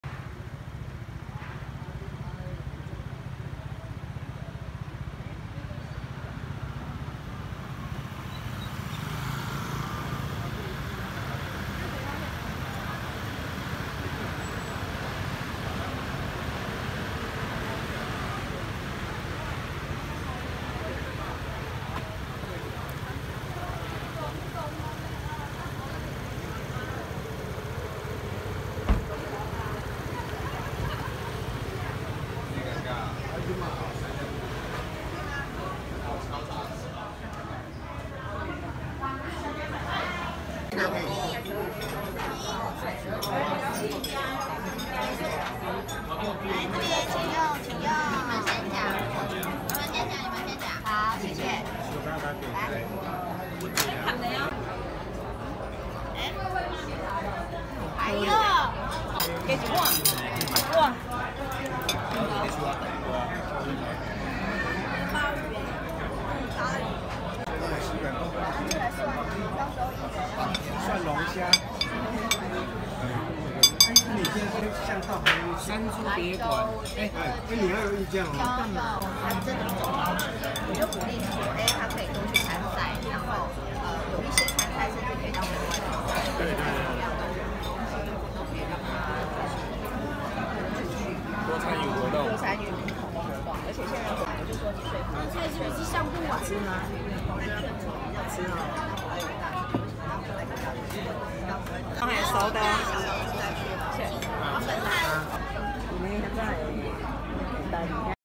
2024-03-16 豐原老字號「圓明園」喜慶廣場 宴席全記錄 Yuan Ming Yuan Relish Restaurant x 雙貝蒸盤/藥燉鰻魚豬腳湯/蒜蓉蒸蝦 x 台中/豐原/圓環東路
豐原 圓明園
沒有華麗的裝潢與優質服務
用一顆對待食物真誠的心，感動您的味蕾

圓明園喜慶廣場～豐原老字號中式宴客餐廳　春酒、尾牙、佳節聚餐、喜宴都適合